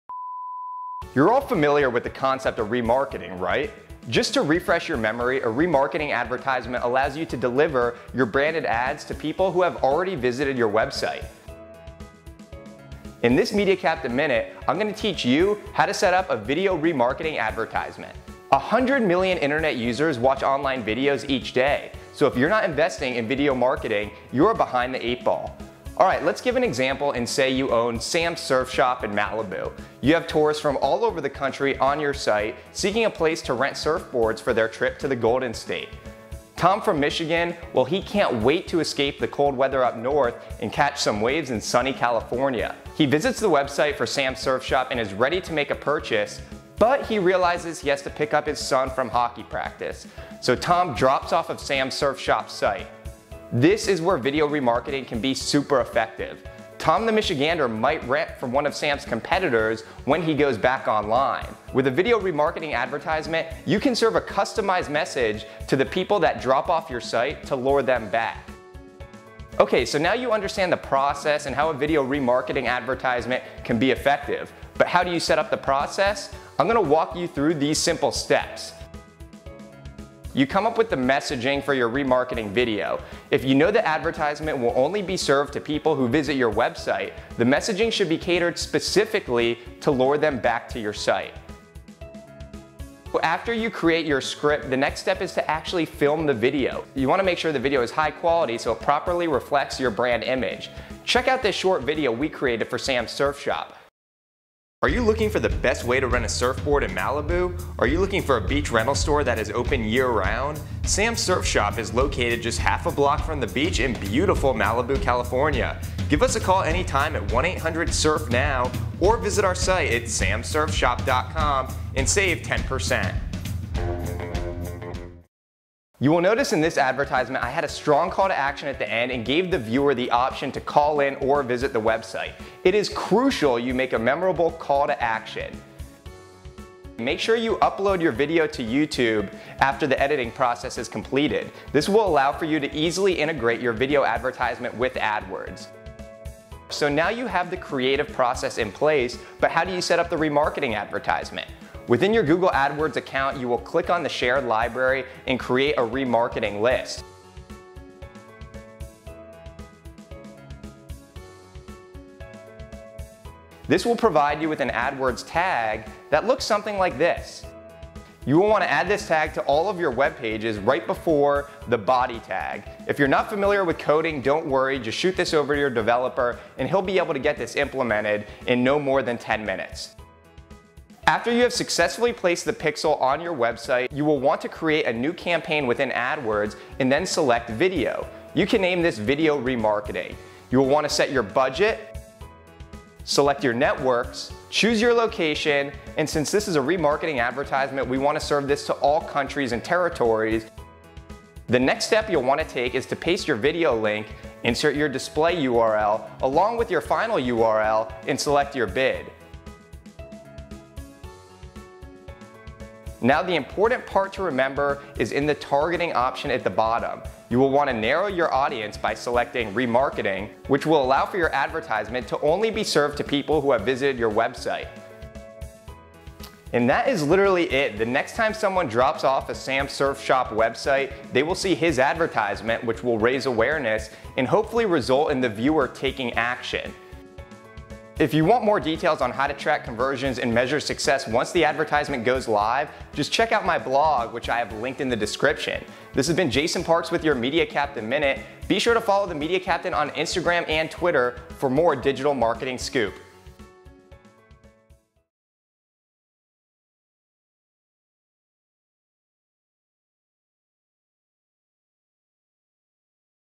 How To Set Up A YouTube Video Remarketing Campaign in Adwords
Why is video remarketing so effective?

100 million Internet users watch online videos each day, so if you are not investing in video remarketing, you are behind the eight ball.

Below is a quick synopsis on how to set-up a YouTube remarketing campaign in Adwords. If you are looking for even more detail, we wrote a blog on this topic:

1. Come up with the messaging for your remarketing video.
2. Create a high quality video
3. Upload your video to YouTube
4. Within your Google Adwords account, click on the shared library and create a remarketing list.
5. Add this tag to all of your web pages, right before the end body tag or in the appropriate place on your site
6.  Create a new campaign within Adwords and then select "video."
7. Set your budget, select your networks, choose your location and serve this to your desired location
8. Paste in your video link, insert your display URL along with your final URL and select your bid.
9. In the targeting option at the bottom, you will want to narrow your audience by selecting “remarketing” which will allow for your advertisement to only be served to people who have visited your website.
10. Launch your campaign and measure your results